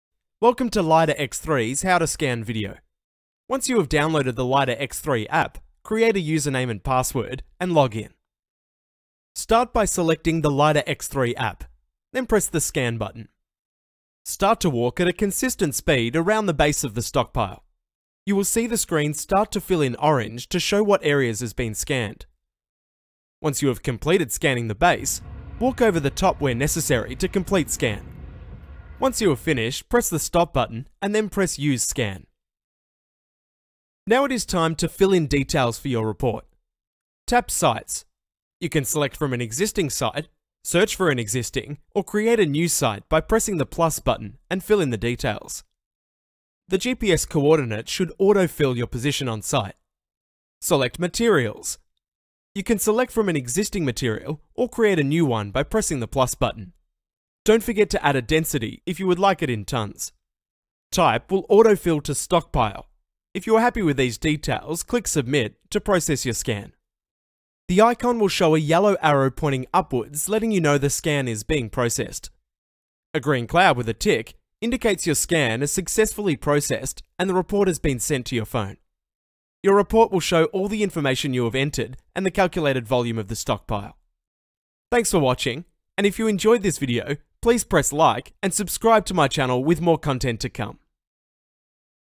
Lidarx3 3d Scanning Stockpiles for Volumes
The Lidarx3 APP will give you the ability to 3D scan your stockpiled materials for volumes and generate a report within minutes, increasing your efficiency in making a decision onsite without delay and all using just your mobile phone and our app.